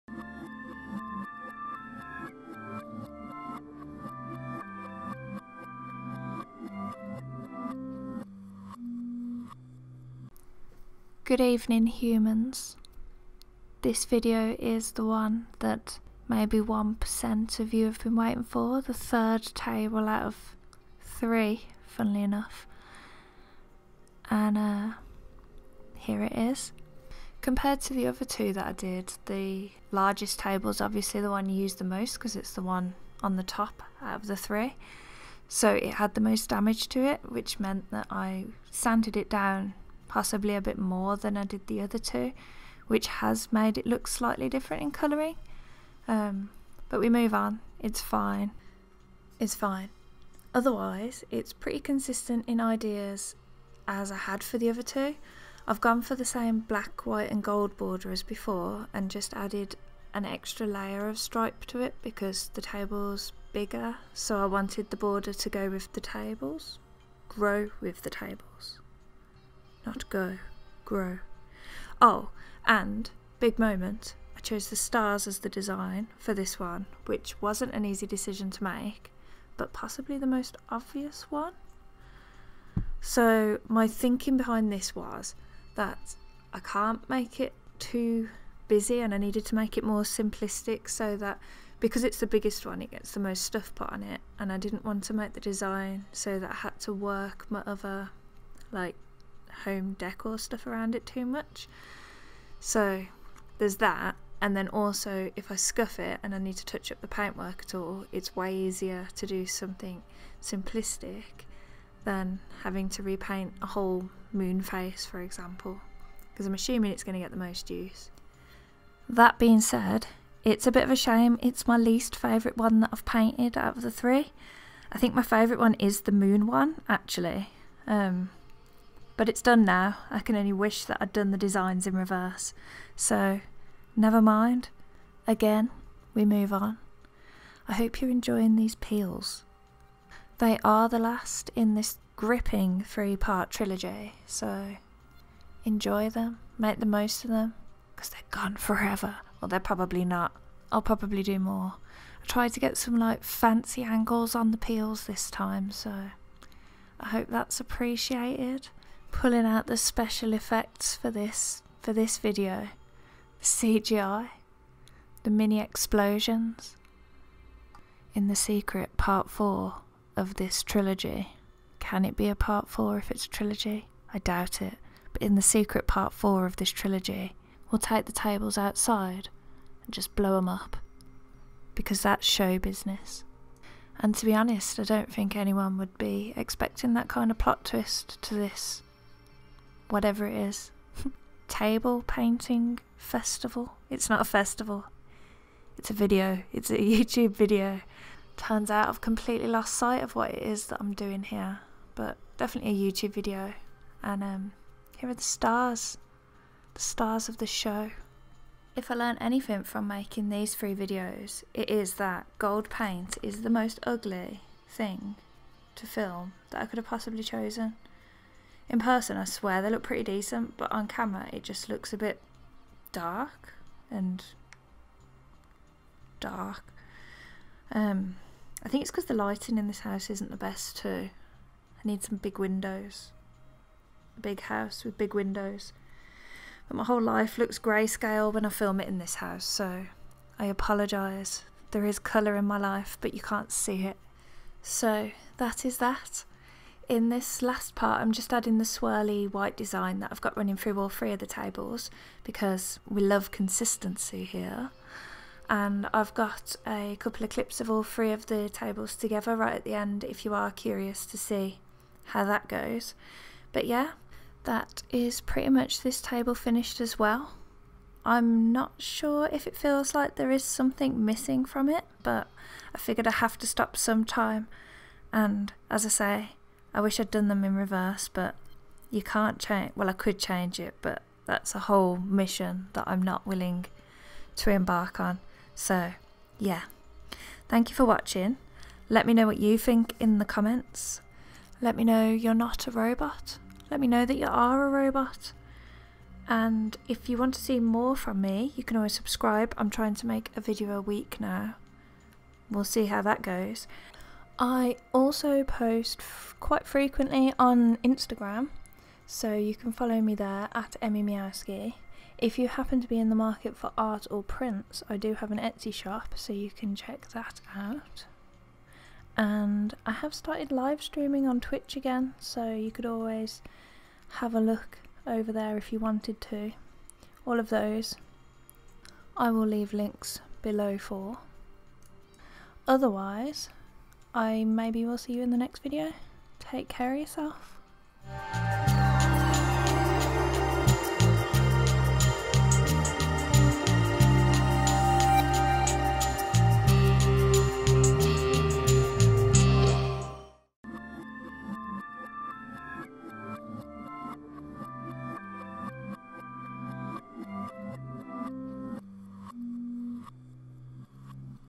PAINTING THE FINAL TABLE - [THE 3RD AND FINAL TABLE!] | Emy Meowski
HELLO CREATURES THE MOMENT WE HAVE ALL BEEN WAITING FOR - TABLE NUMBER 3. THE GRAND FINALE IN THE NESTING TABLE SERIES - KEEPING YOU ON THE EDGE OF YOUR SEATS.

If you missed the first 2 table paintings in the series you can WATCH

Sundial